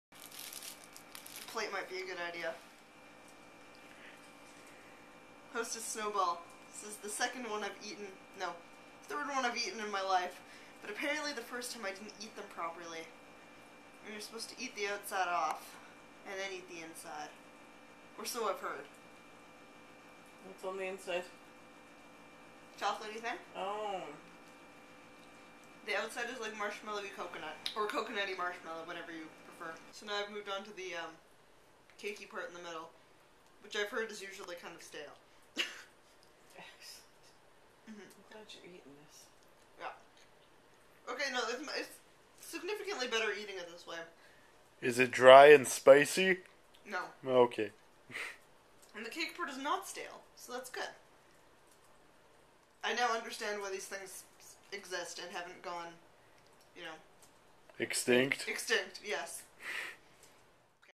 hostess snowball
One time in Orlando I had a Hostess Snowball and it was gross.  Because I found out that I was eating it wrong apparently.

So, the Snowball and I got reacquainted after I learned how after that event.  Ignore my pool/shower hair and whatnot.

Yeah, I actually liked it.